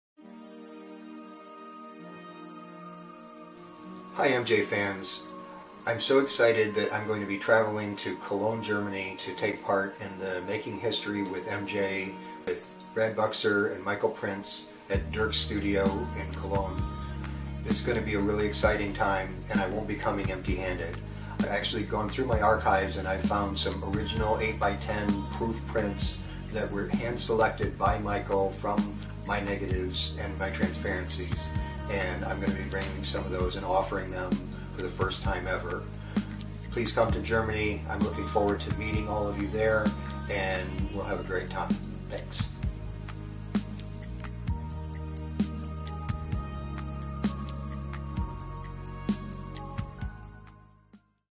Steven Whitsitt - Video message for Making HIStory with MJ, Cologne
A message video from Steven Whitsitt, Michael Jackson photographer during HIStory era. Join him at "Making HIStory with MJ" in Cologne, June 25 & 26.